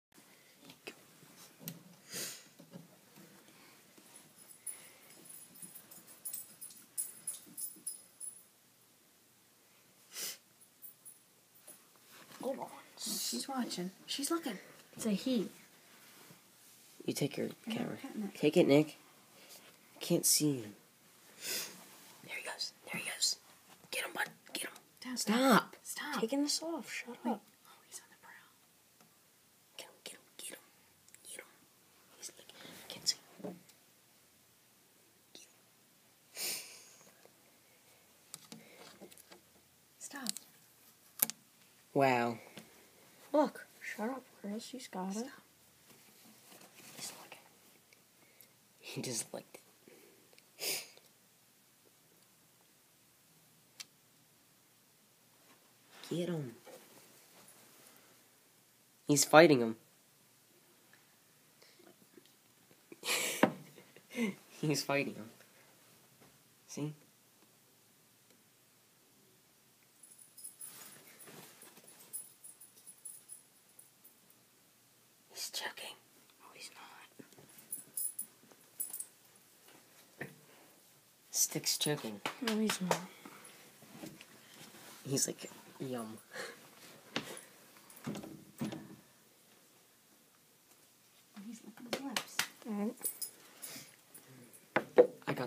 Leopard Gecko eats tomato worm
Stick the lizard eats a blue tomato worm.